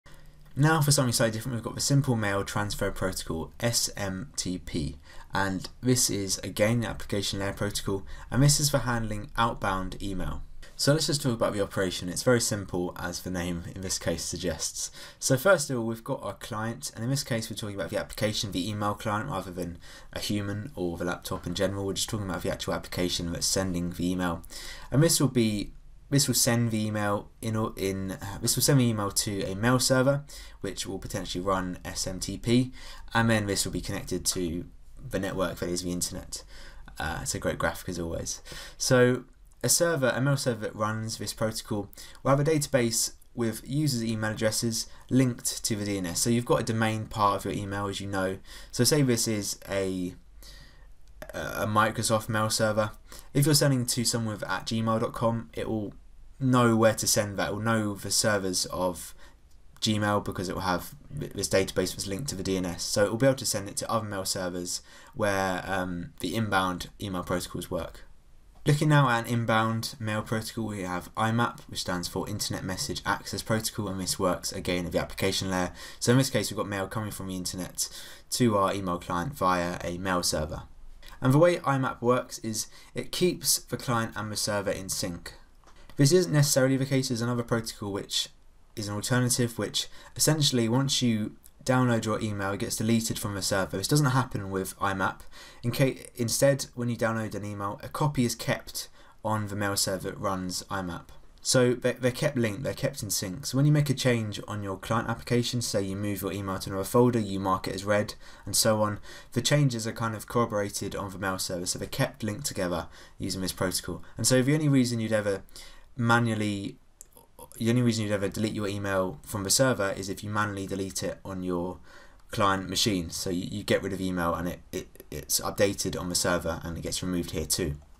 Simple Mail Transfer Protocol (SMTP) and Internet Message Access Protocol (IMAP)
Covering 2 main application layer email protocols, SMTP and IMAP. Made specifically for AQA GCSE Computer Science students.

These videos will always be free but if you'd consider a donation I'd be extremely grateful